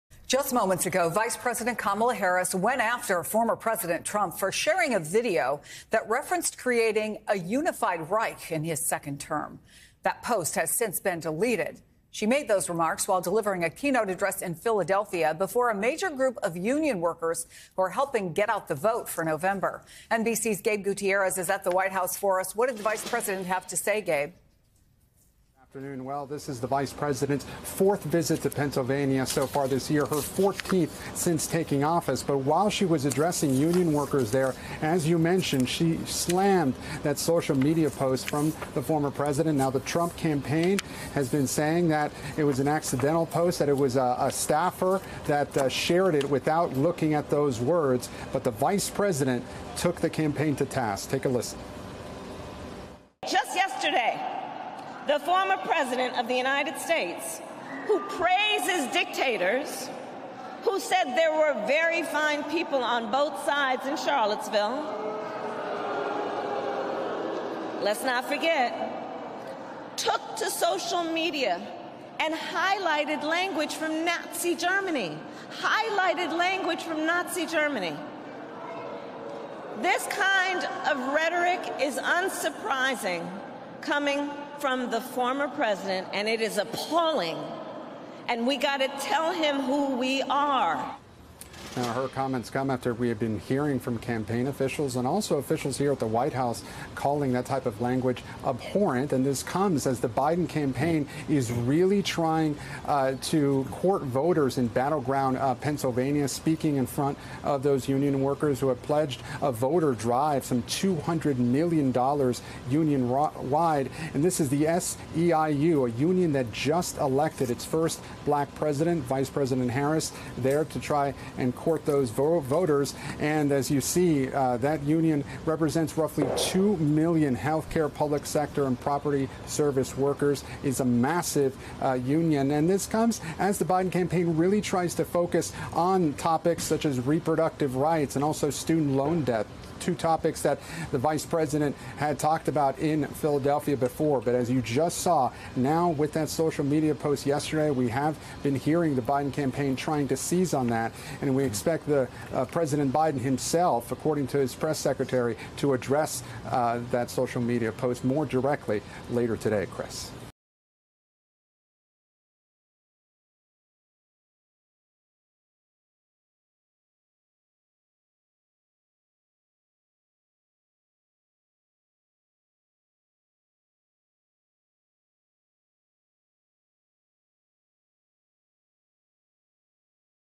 Harris criticizes Trump for promoting video referencing ‘unified Reich’
Vice President Kamala Harris criticized former President Donald Trump after his social media account shared a controversial post that has since been deleted. NBC News’ Gabe Gutierrez reports on the fallout after Trump’s account shared a video referring to a ‘unified Reich’ among possible outcomes if he were to win re-election in November.